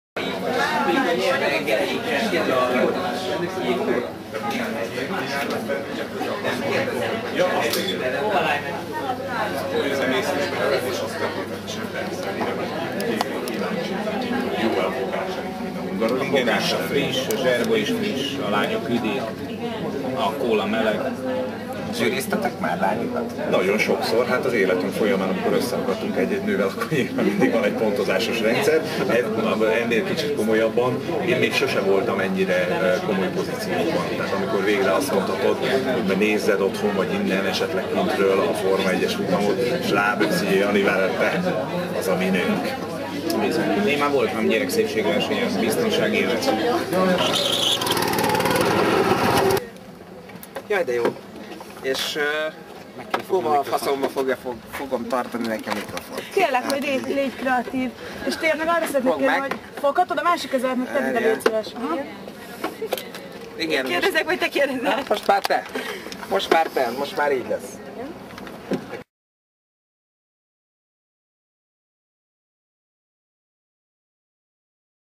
Grid Gril választás.wmv
Vadon Jani és Rákoczi Feri Class FM választottja "rajtrács cica" Budaházi Nóra